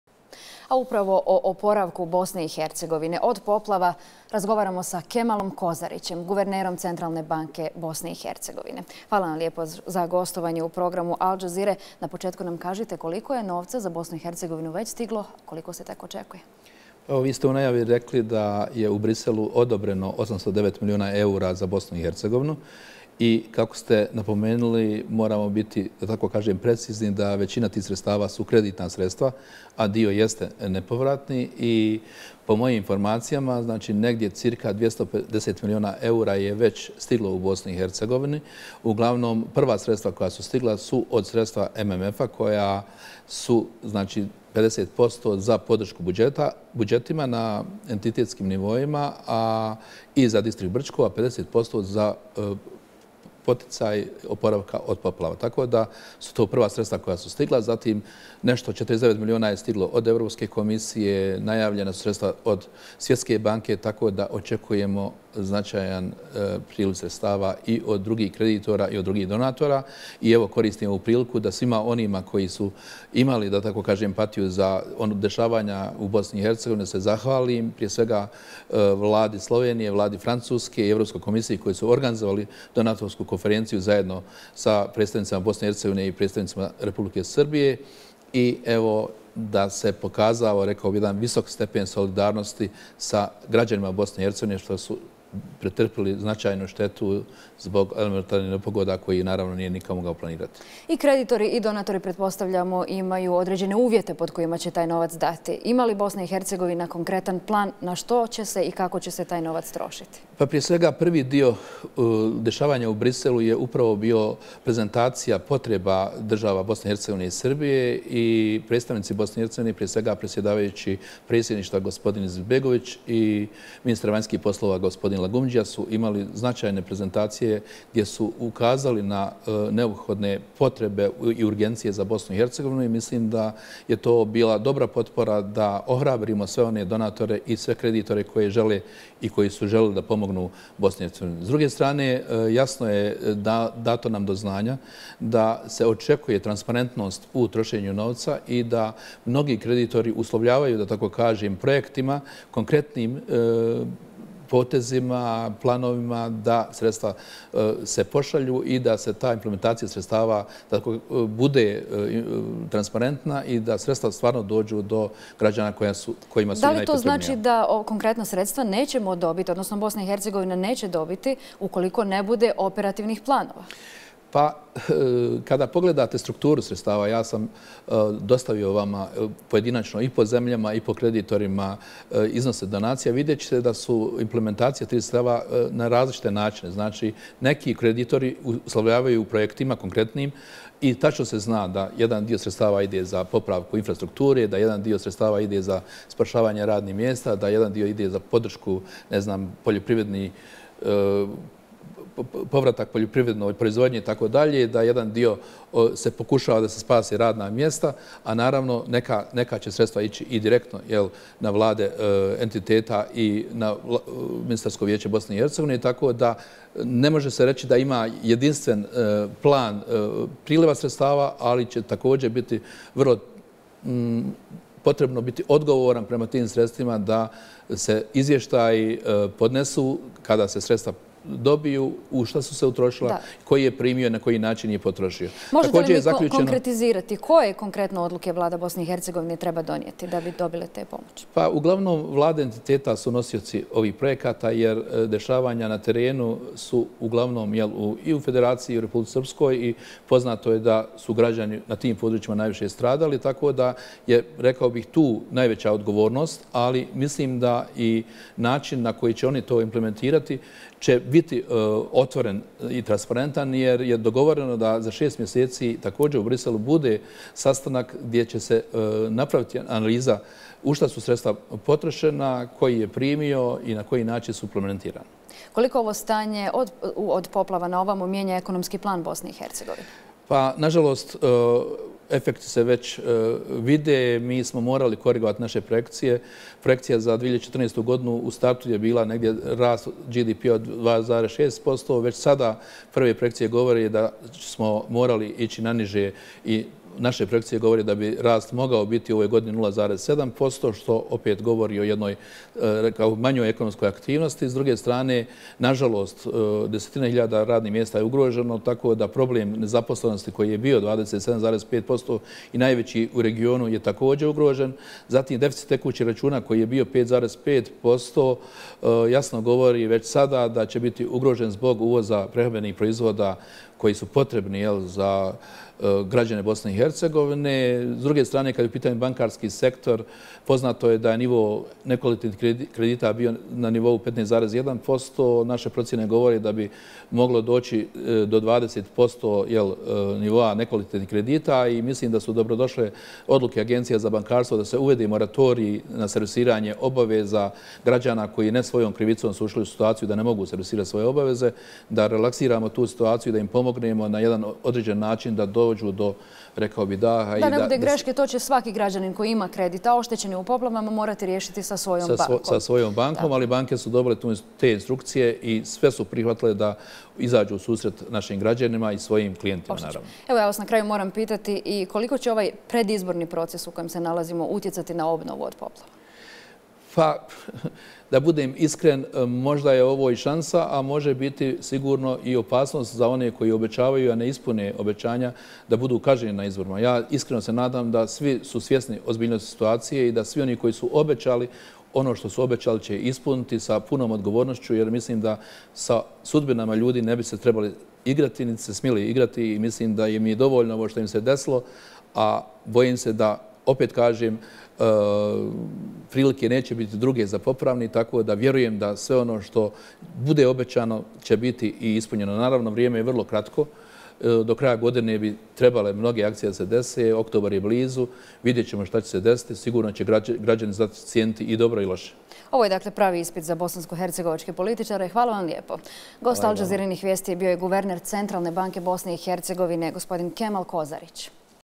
Kemal Kozarić o oporavku BiH od poplava
Na nedavno održanoj donatorskoj konferenciji u Briselu, 16. jula, za pomoć poplavljenim područjima Srbije i Bosne i Hercegovine, zemljama je na raspolaganje stavljeno ukupno milijardu i 795 miliona eura.